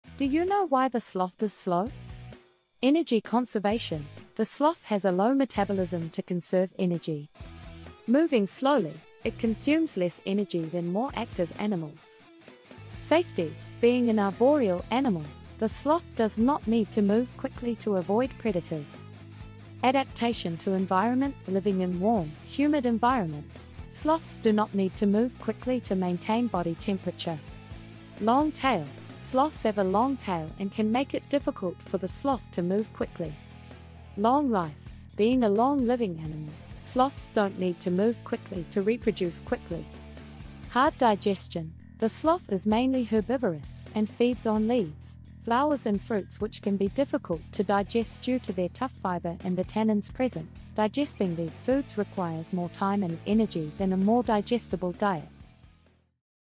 Why the Sloth is So SLOW - Big Fire
do you know why the sloth is slow?,The sloth is slow because it has a low metabolism, which allows it to conserve energy. There are several hypotheses as to why the sloth is so slow. In addition to the low calorie content of his diet, some of the reasons could be:

Energy Conservation: As already mentioned, the sloth has a low metabolism to conserve energy. Moving slowly, it consumes less energy than more active animals.

Safety: being an arboreal animal, the sloth does not need to move quickly to avoid predators. Most of the sloth's predators are land animals, and sloths can simply climb trees to safety.

Adaptation to environment: Living in warm, humid environments, sloths do not need to move quickly to maintain body temperature.

Long Tail: Sloths have a long tail which they use to maintain balance as they move through the trees. However, this tail can make it difficult for the sloth to move quickly.

Long life: Being a long-living animal, sloths don't need to move quickly to reproduce quickly.

In general, the slow gait of the sloth is an adaptation to its sedentary arboreal life and the scarcity of food in the environment in which it lives, and the combination of these factors make the slow gait an advantageous choice for the survival of the species.